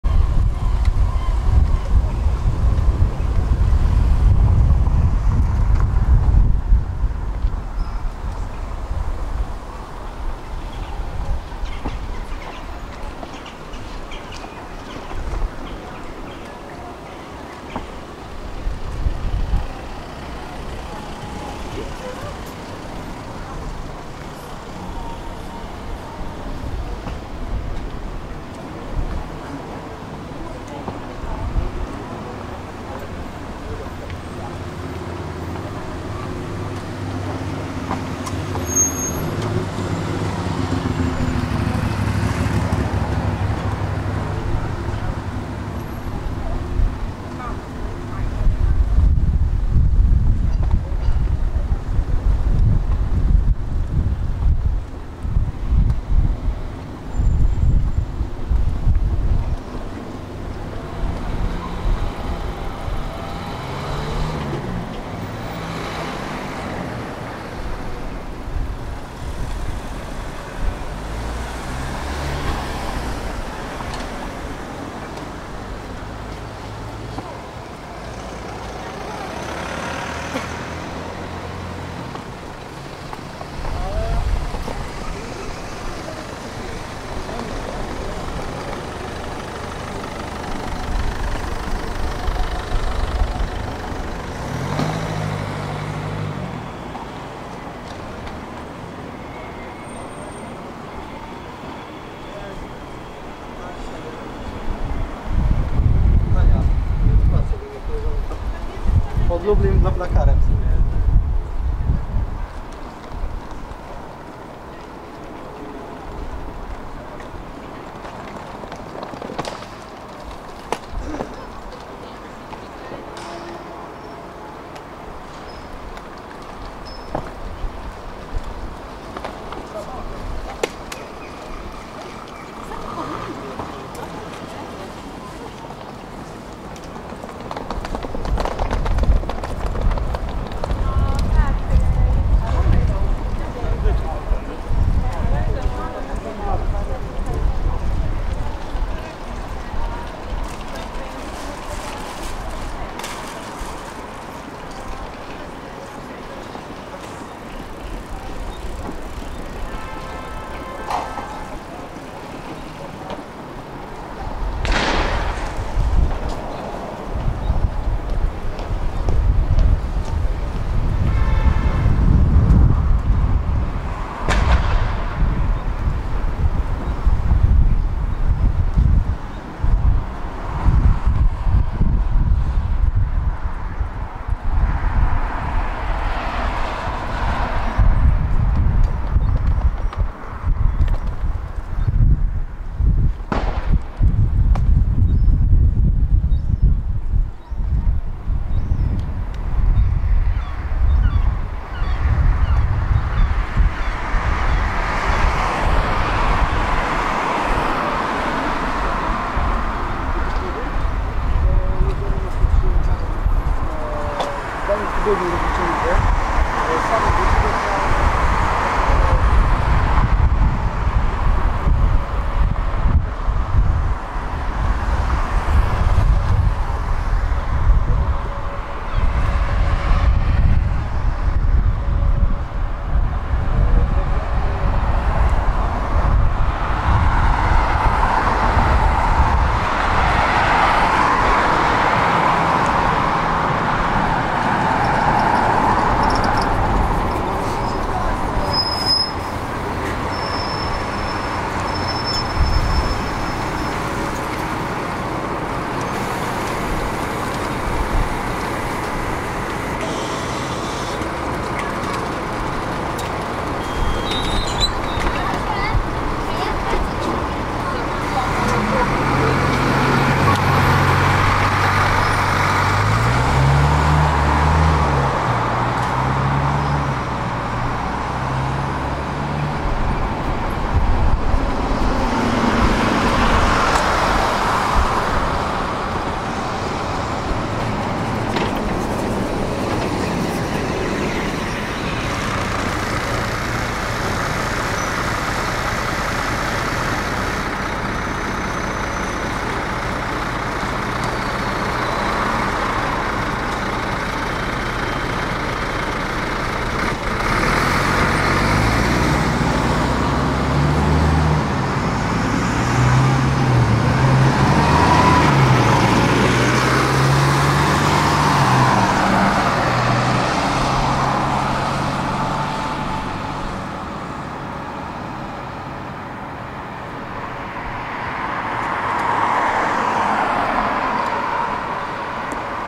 Poland, Gdańsk, short summer walk to bus stop Dworzec Główny PKP
August 2022